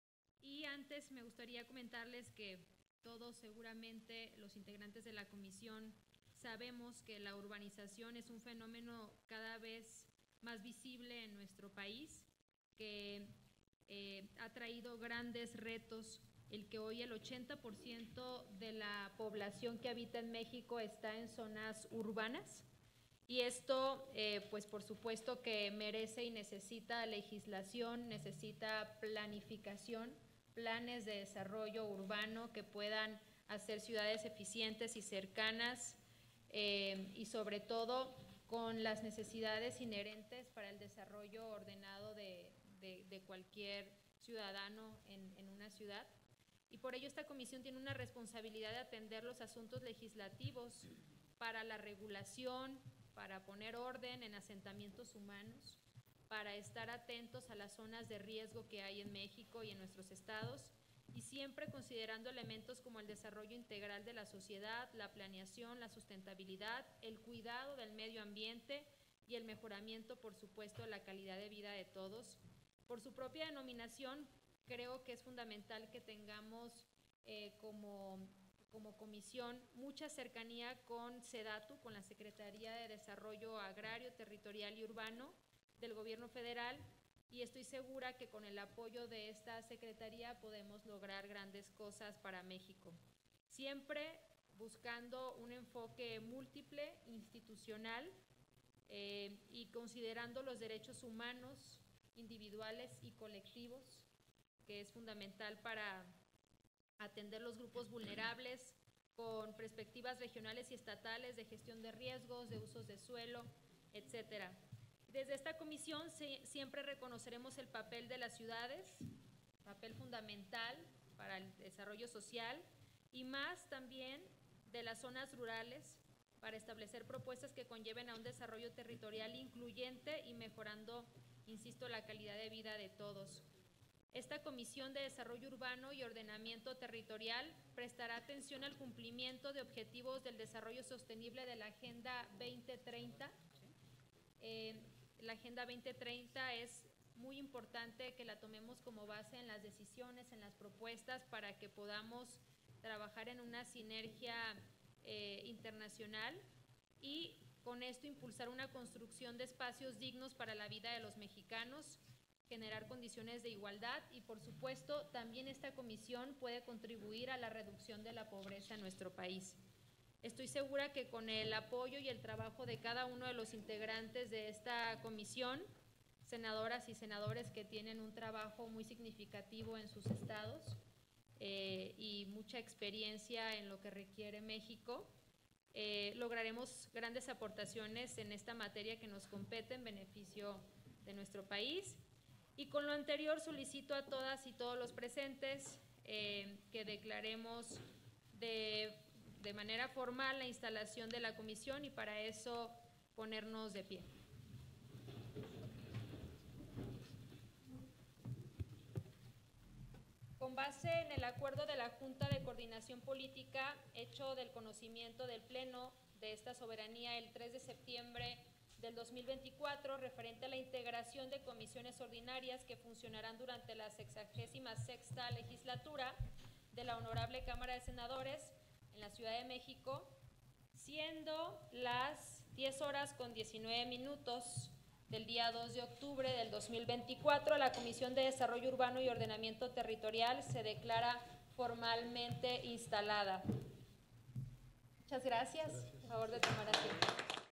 Senadora Susana Zatarain: instalación de la Comisión de Desarrollo Urbano y Ordenamiento Territorial
2 de octubre de 2024.- Intervención de la senadora Susana Zatarain García, en la reunión de instalación de la Comisión de Desarrollo Urbano y Ordenamiento Territorial.